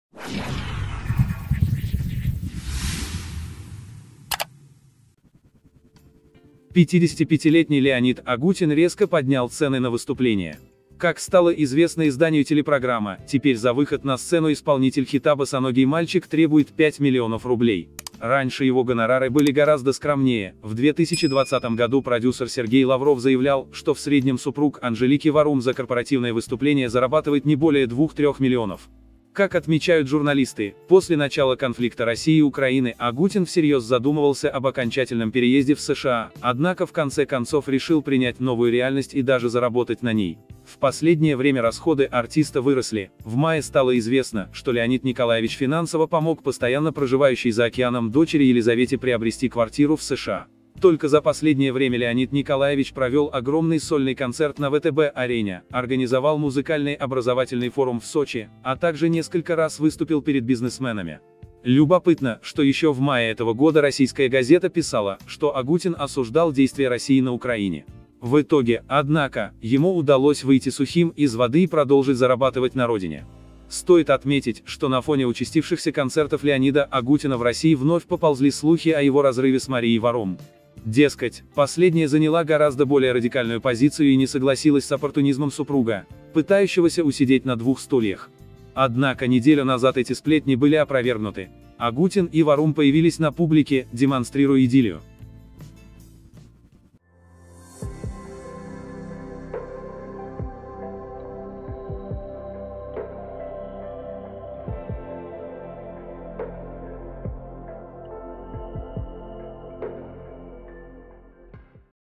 Леонид Агутин взвинтил цены за выступления
Скупающий недвижимость в США Леонид Агутин взвинтил цены за выступления.